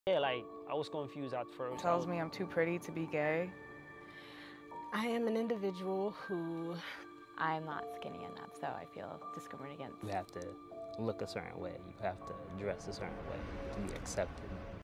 DLM "Don't Label Me " campaign
We decided to shoot DLM "Don't Label Me " campaign to cast a vivid light on things said and done that sometimes hurt a person and evidently seeps into their long term memory, holding onto that discriminating or judgmental comment.

What we discovered was that even the things that someone might consider innocent can infect the most damage.

Jessica, co-producer,

POV on the episode : During Don't Label Me, I was sitting watching the person get interviewed, along with the journalist, producer, co-producer, crew, and a couple of other people waiting to be interviewed. I could feel the energy shift directly onto the person being interviewed once each started. I'm sitting there, drowning with empathy, and tears in my eyes, as these people answer these questions with pure honesty, and genuinely. And I couldn't help but feel honored to be sitting in that room, listening and witnessing that person open their heart in our studio.